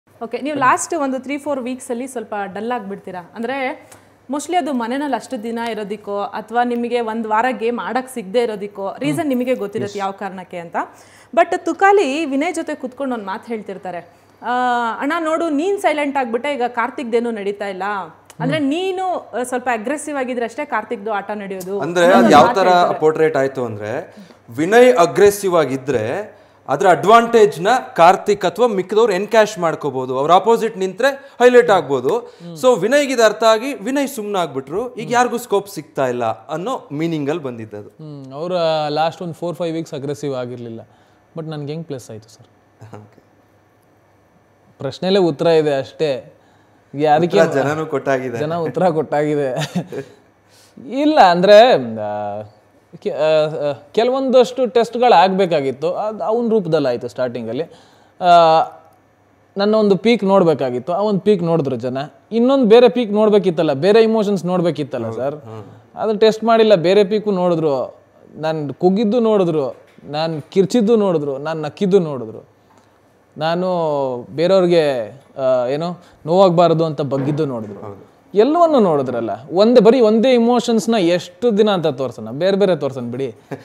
Tukali Santhosh ಹೇಳ್ದಂಗೆ Vinay Gowda ಅಗ್ರೆಸಿವ್ ಆದ್ರಷ್ಟೇ ನೀವುಆಟ ಆಡ್ತಿದ್ರಾ..? | Bigg Boss 10 Winner
Tukali Santhosh ಹೇಳ್ದಂಗೆ Vinay Gowda ಅಗ್ರೆಸಿವ್ ಆದ್ರಷ್ಟೇ ನೀವುಆಟ ಆಡ್ತಿದ್ರಾ..? | Bigg Boss Kannada 10 Winner | FilmyFirst Kannada